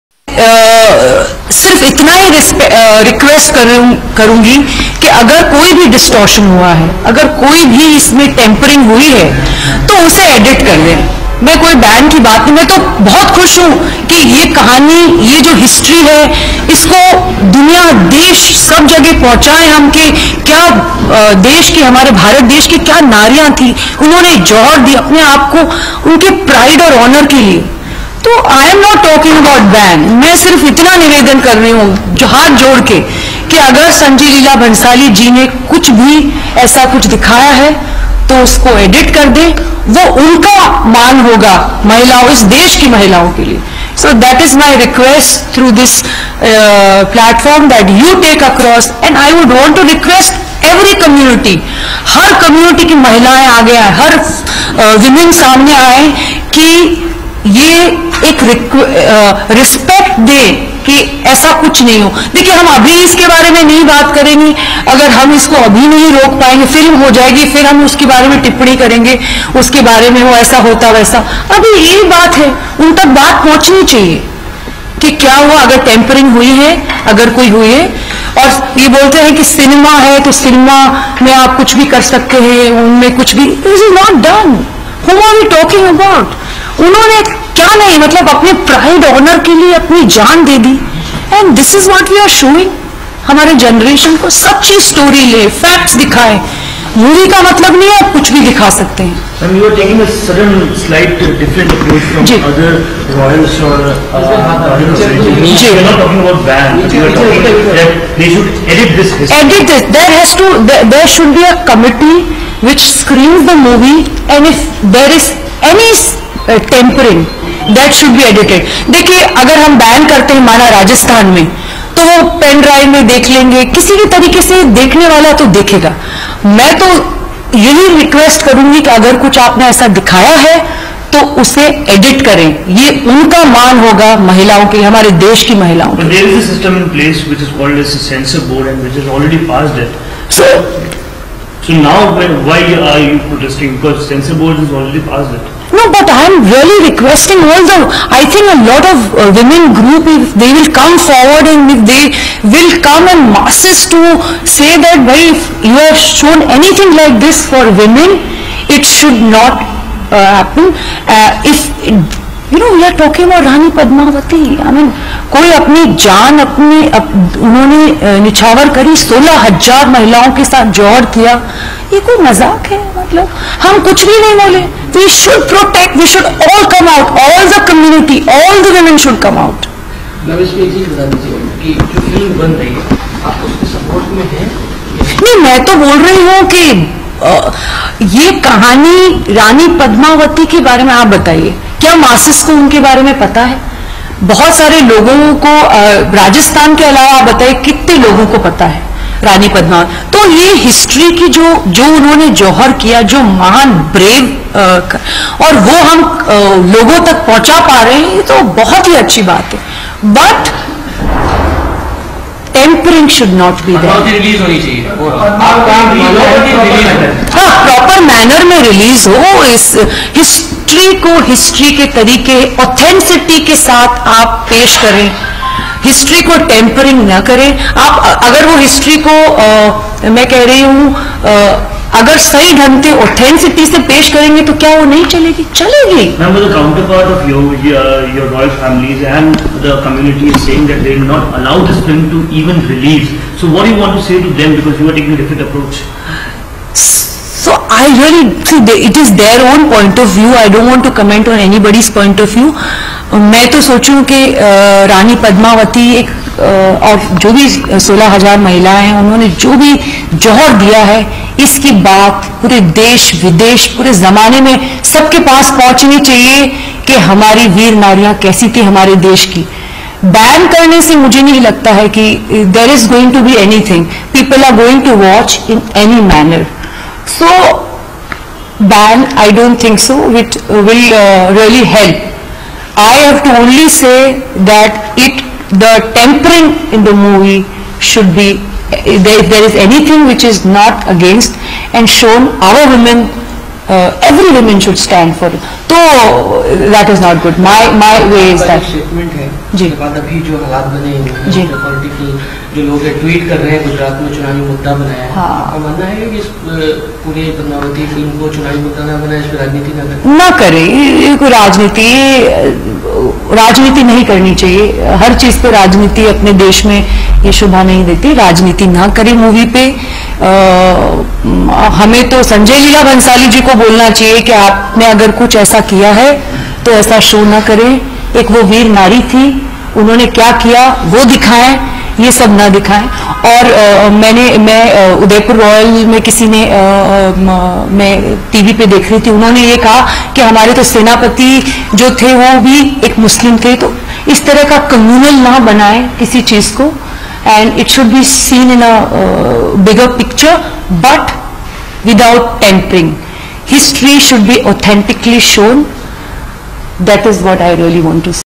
Rukshmani Kumari Royal Family of Chomu Jaipur on Padmavati 08 11 2017
Princess Rukshmani  Kumari 's  Media Conference  on Sanjay Leela Bansali Film Padmavati

."History has to be protected an specially when it is about women"
" It's is completely unnecessary to hurt sentiments,distort history in the name of cinematic liberty"